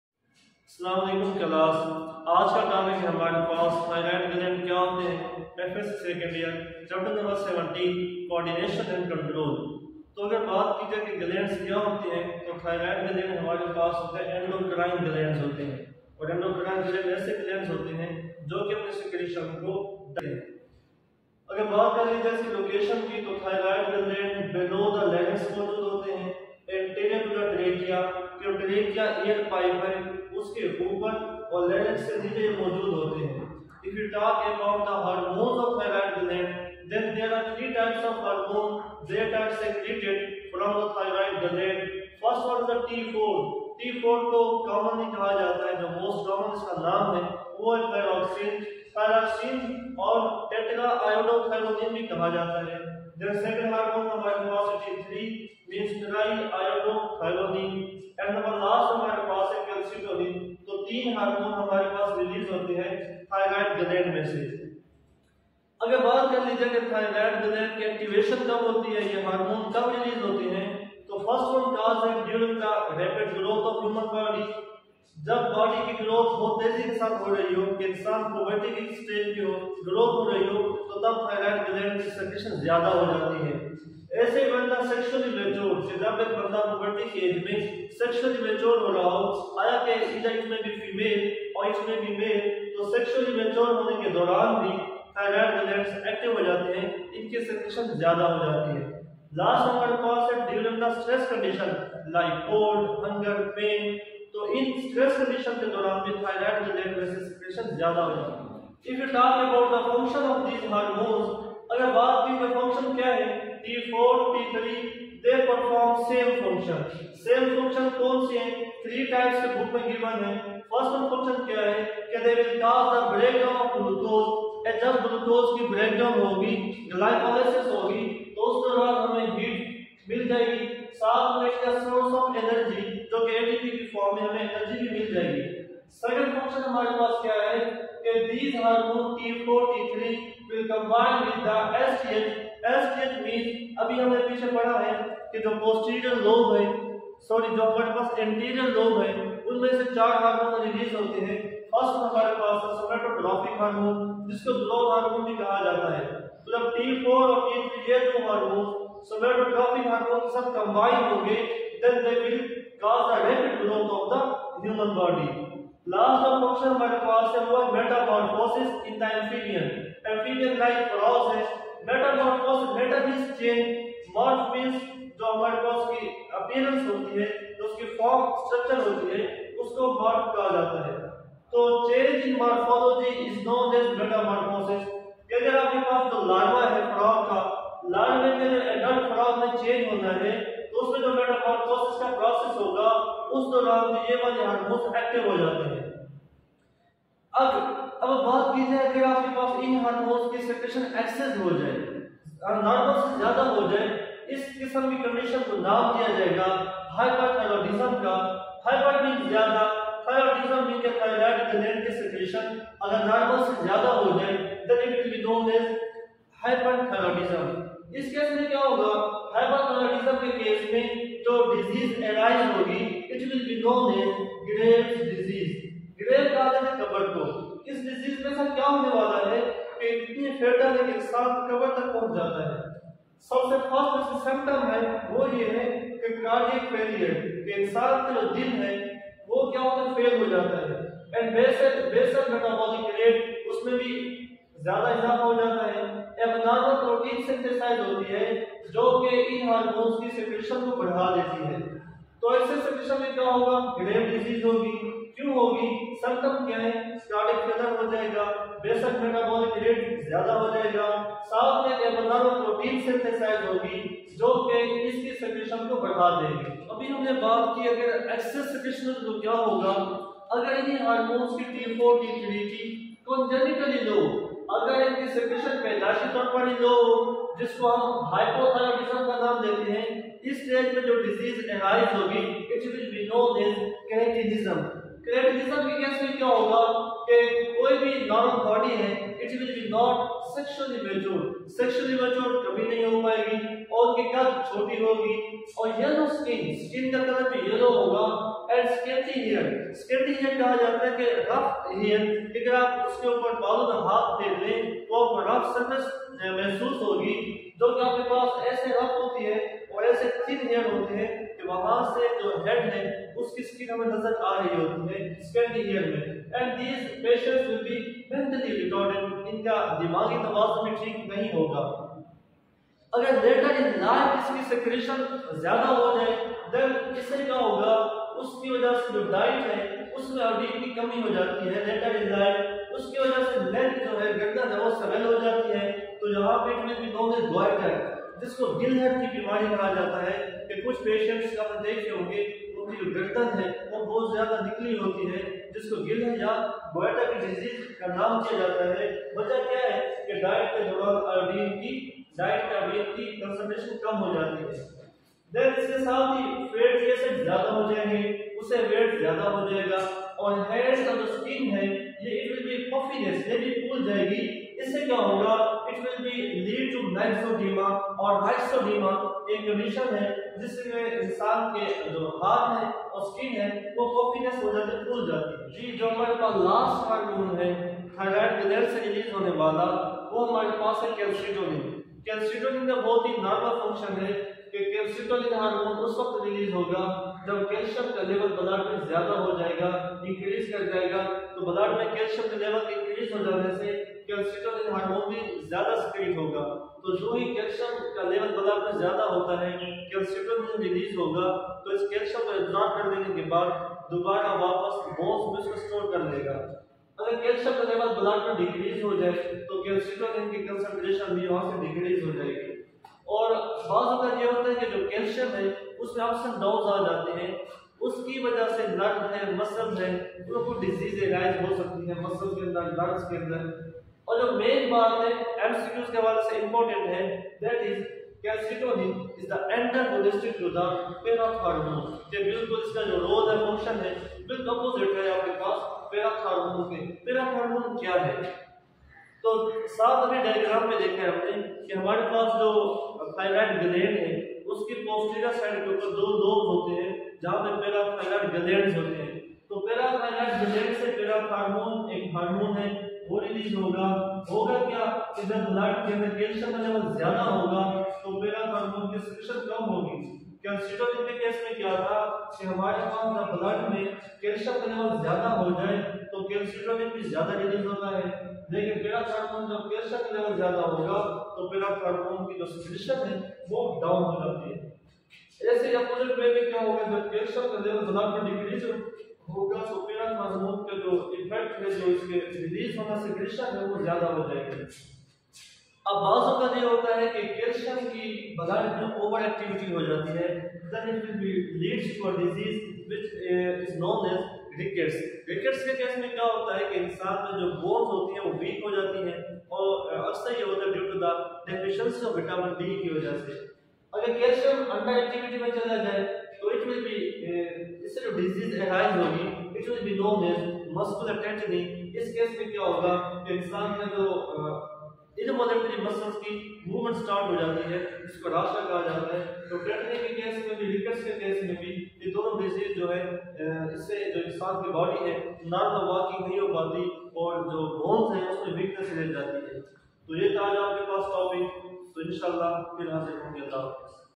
Thyroid gland Anatomy || F.Sc 2nd year Biology. Chapter 17#Coordination and control.
in this topic we will discuss about endocrine glands thyroid glands the function of thyroid glands the types of thyroid glands.
we will discuss about the various diseases like cretinism and goitre and maxodema.
we will also discuss about the function of parathyroid glands and parasharmons and its roles in the body.